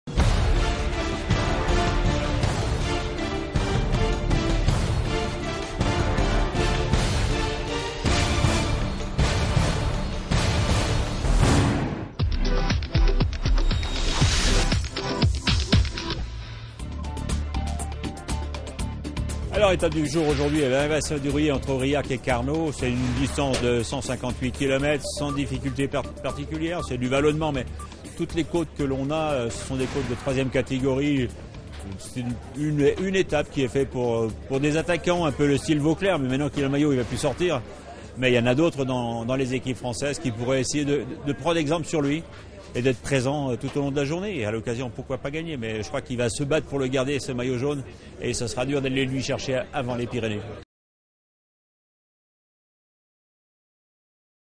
ÉTAPE 10 - Analyse de l'étape
Chaque matin avant le départ, Bernard Hinault donne son sentiment sur les enjeux du jour, les coureurs à observer sur l'étape.